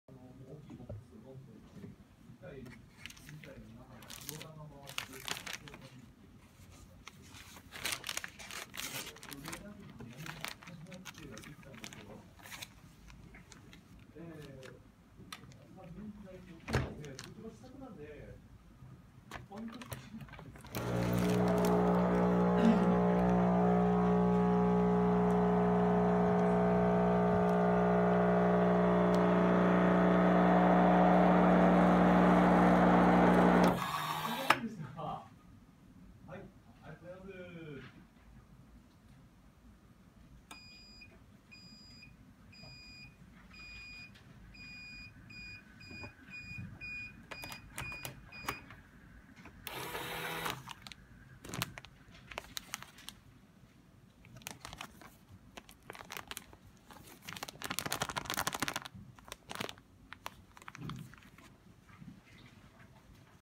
おむつ～真空パック機 吸太郎～
真空パック器　吸太郎でおむつを真空パックしてみました。
かさばるオムツがぺしゃんこに。